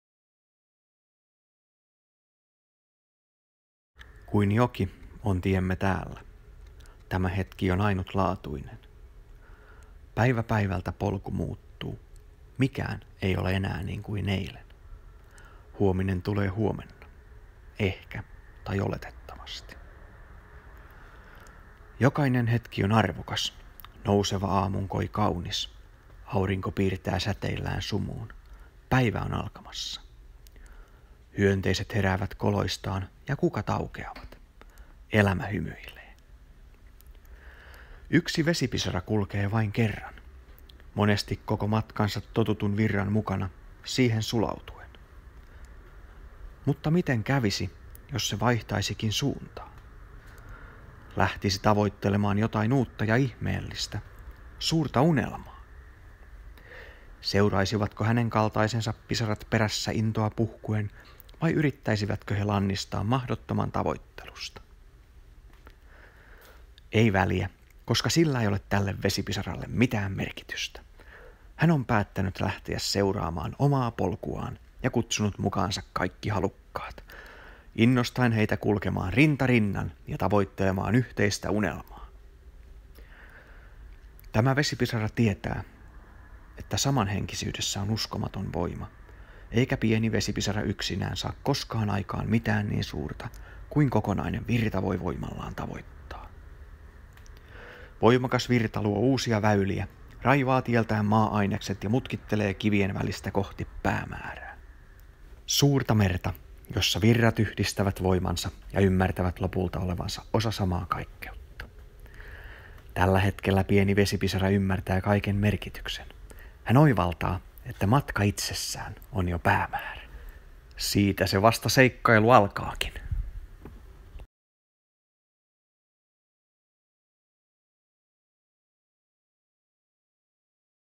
Pieni vesipisara
Kertomus ajasta, elämästä ja pienestä vesipisarasta.

-Toisinaan sitä syntyy jotain, mitä itsekin voi vain ihmetellä :)